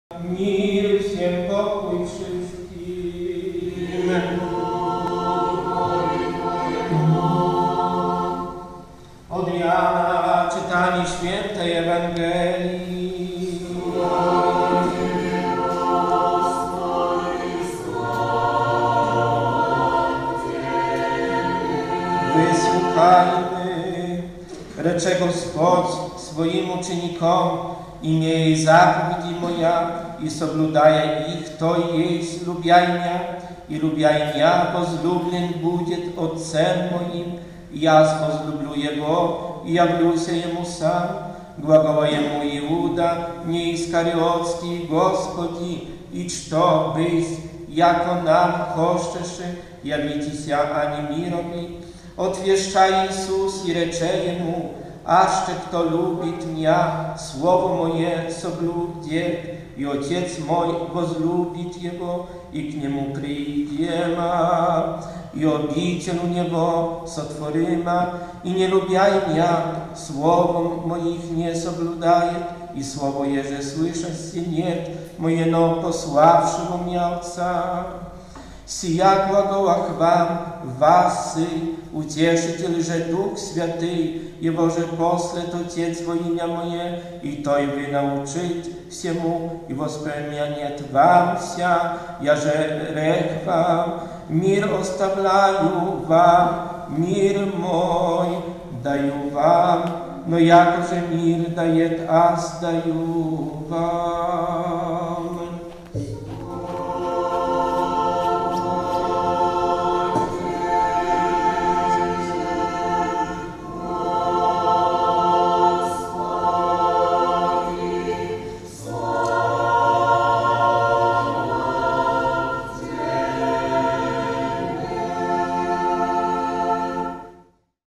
Fragment Ewangelii św. Jana czyta ks. Eugeniusz Fiedorczuk
Kilkadziesiąt osób wzięło udział 17 stycznia w nabożeństwie w łódzkiej katedrze prawosławnej św. Aleksandra Newskiego. Rozpoczęło ono obchody Tygodnia Modlitw o Jedność Chrześcijan w Łodzi, Pabianicach i Zgierzu. Przed tamtejszym ikonostasem wspólnie modlili się kapłani Kościołów: prawosławnego, ewangelicko-reformowanego, rzymskokatolickiego, polskokatolickiego, starokatolickiego mariawitów, ewangelicko-augsburskiego, ewangelicko-metodystycznego i chrześcijan baptystów.